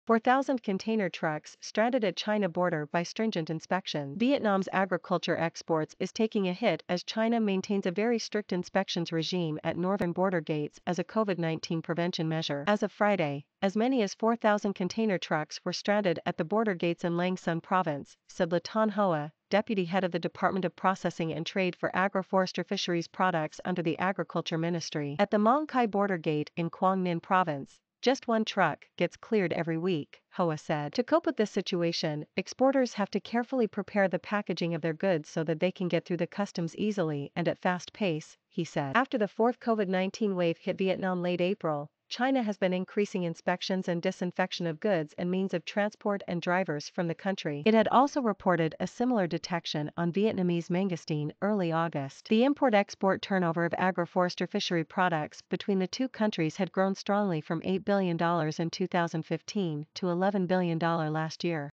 4,000 container trucks stranded at China border by stringent inspections
Vietnam's agriculture exports is taking a hit as China maintains a very strict inspections regime at northern border gates as a Covid-19 prevention measure. As of Friday, as many as 4,000 container trucks were stranded at the border gates in Lang Son Province, said Le Thanh Hoa, deputy head of the Department of Processing and Trade for Agro-Forestry-Fisheries products under the agriculture ministry.At the three border gates of Huu Nghi, Tan Thanh and Chi Ma, the current customs clearance speed has fallen by more than half to about 220 container trucks per day, Hoa said at a conference on agriculture trade held Saturday in Hanoi.For each truck carrying dragon fruit, jackfruit and other fruits stuck at the Tan Thanh gate, it was taking 10-14 days on average to get customs clearance.At the Mong Cai border gate in Quang Ninh Province, just one truck gets cleared every week, Hoa said.The management board of the Mong Cai gate said Saturday morning that as many 800 trucks of frozen seafood and 300 trucks of fruits were stranded there.Hoa recommended that businesses carefully check agricultural products exported via border gates because China has been strengthening its Covid-19 disease control regime, resulting in careful, stringent inspections of products entering its territory.To cope with this situation, exporters have to carefully prepare the packaging of their goods so that they can get through the customs easily and at fast pace, he said.On the other hand, to avoid congestion and save costs, businesses need to arrange a reasonable clearance time and not send many trucks to the border gates at the same time, he added.After the fourth Covid-19 wave hit Vietnam late April, China has been increasing inspections and disinfection of goods and means of transport and drivers from the country.They are also tightening the management and traceability of goods, which has contributed to lengthening the customs clearance procedure.In September, China had temporarily ceased the import of dragon fruit from Vietnam after detecting the novel coronavirus on its packaging. It had also reported a similar detection on Vietnamese mangosteen early August.Hu Suo Jin, Economic and Commercial Counselor of the Chinese Embassy in Vietnam, said the pandemic in Vietnam was developing quite complicatedly and exporters need to disinfect the means of transport and drivers also need to increase adoption of preventive measures to avoid leaving the virus on goods.In addition, if possible, Vietnamese goods should be labeled with a negative test certificate on their packaging, he said.China is one of Vietnam's top trading partners. The import-export turnover of agro-forestry-fishery products between the two countries had grown strongly from $8 billion in 2015 to $11 billion last year.China was Vietnam's second-largest export market for agricultural, forestry and fishery products behind the U.S., posting an export turnover of $8.4 billion in the first 11 months of the year, accounting for 19.2 percent of Vietnam’s total agricultural exports.